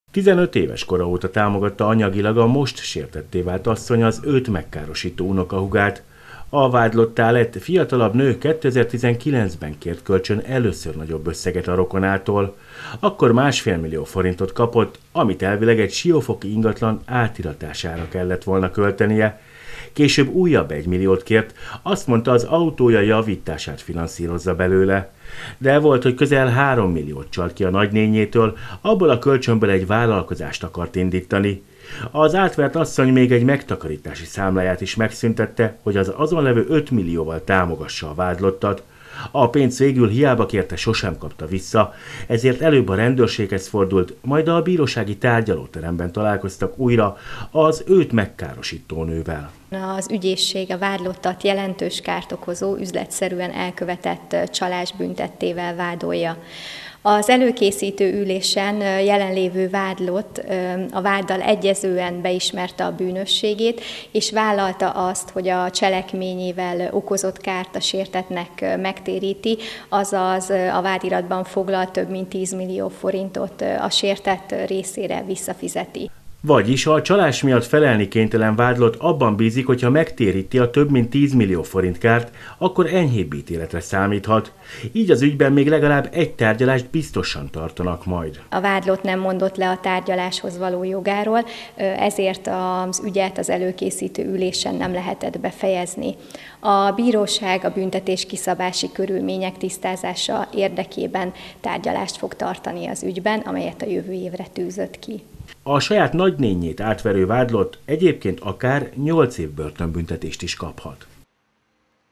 Saját rokonát verte át milliókkal - gyerekkora óta segítette az áldozat az elkövetőt
Több mint 10 millió forinttal károsította meg saját nagynénijét egy nő. Több részletben kért tőle pénzt. Volt, hogy azt mondta, ingatlan vásárlásra kell, vagy éppen az autója szervizelésére. Az áldozat még azt a számlát is kiürítette, amin a megtakarítását tartotta, csak hogy segítsen az őt átverő rokonnak. A kölcsön viszont soha nem lett kiegyenlítve. Az ügyből ezért bírósági rendőrségi nyomozás, majd bírósági eljárás lett.